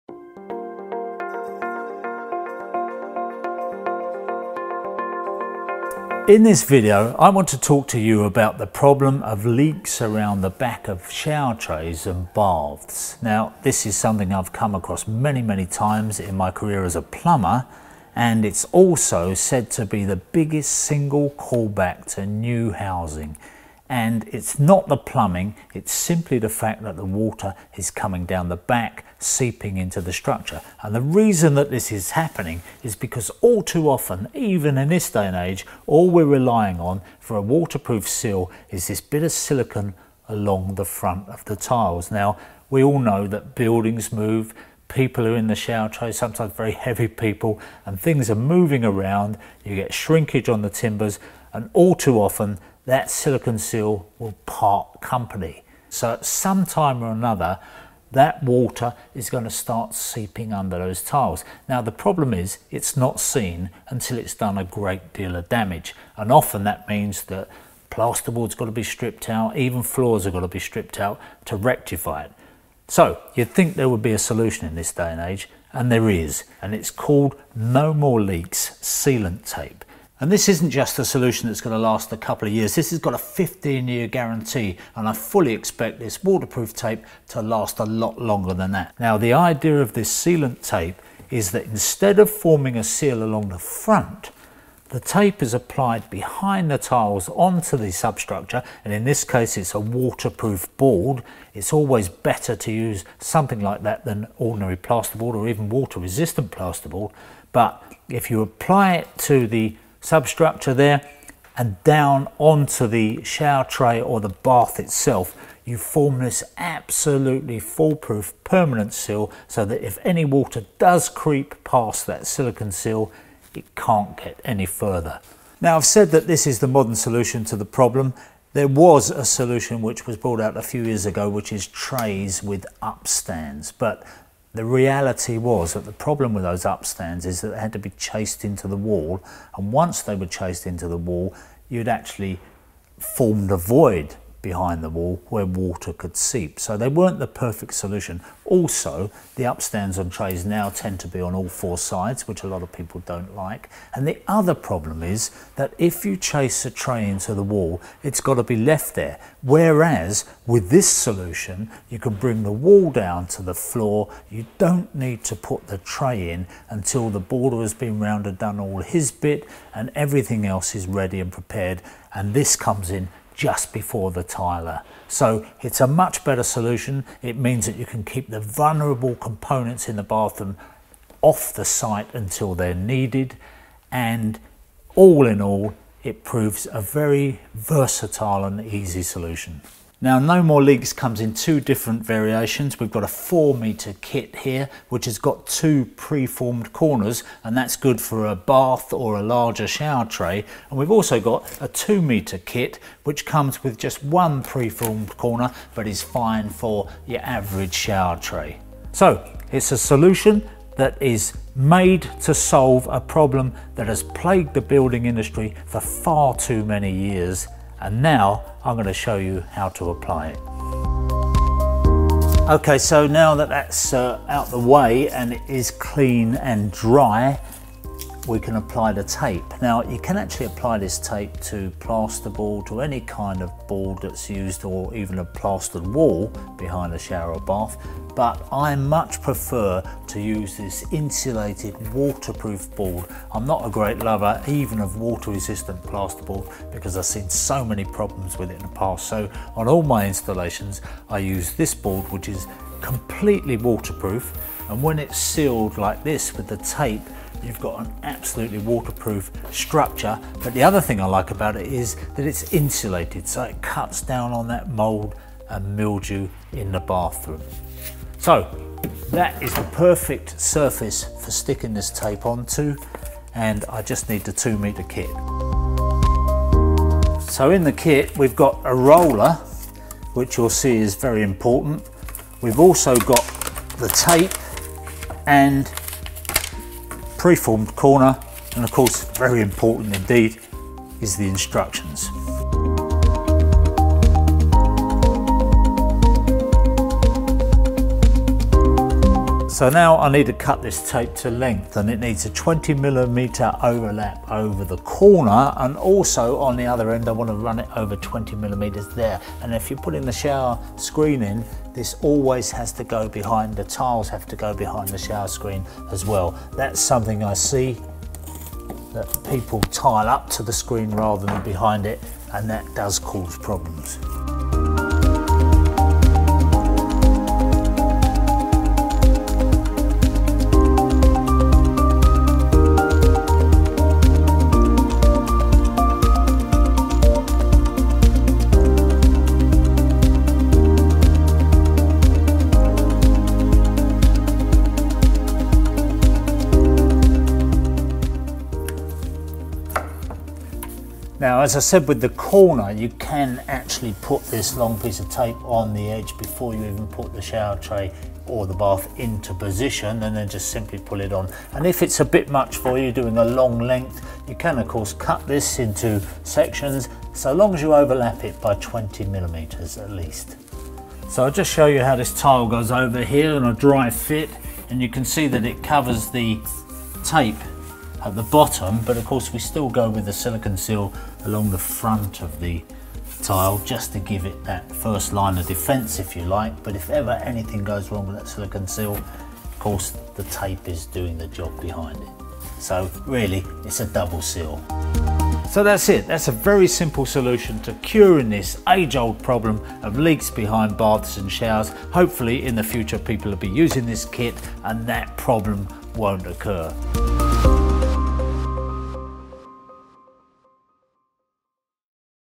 No More Leaks Bath & Shower Sealing Kit
Roger Bisby from Skill Builder shows you how to get the most from the new "No More Leaks Bath & Shower Sealing Kit" by Abacus Manufacturing Group.

The Sealing Kit forms a waterproof seal along the edges of shower trays and baths, making the No More Leaks Bath & Shower Sealing Kit the perfect waterproofing solution for anyone purchasing a new bath or shower tray.

Kits include either 2m or 4m of self-adhesive waterproofing tape, 2 pre-formed internal corners - for ease of installation, ensuring integrity of waterproof seal and a handy roller to ensure the perfect finish when applying the tape.

The 4m Kit is suitable for sealing around baths and larger shower trays, while the mini (2m) kit is perfect for smaller quadrant shower trays.

Offers complete peace of mind when installing new baths or shower trays – No More Leaks!

Stay up to date with Abacus Manufacturing Group!